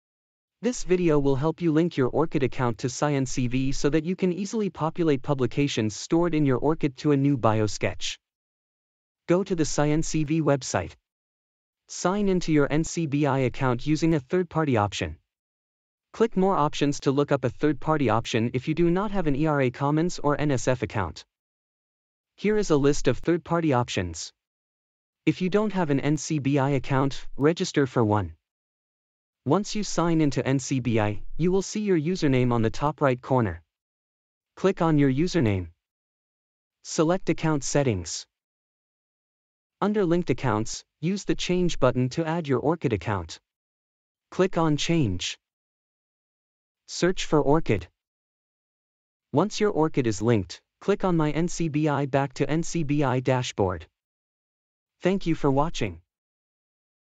Part 1: Link ORCiD to SciENcv
The video will help you link your ORCiD account to SciENcv so that you can easily populate publications stored in your ORCiD to a new biosketch.